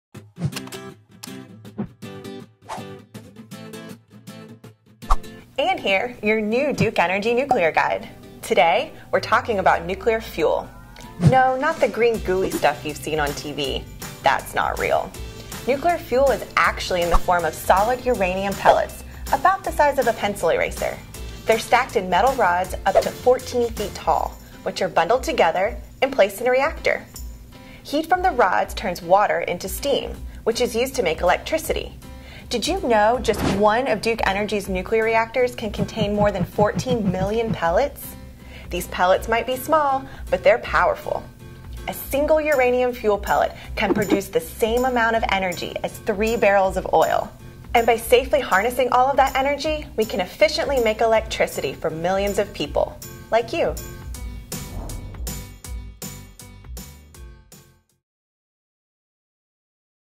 Nuclear Energy in 60 Seconds: Nuclear Fuel
One uranium fuel pellet can contain as much energy as three barrels of oil. Learn more about nuclear fuel from our nuclear energy guide.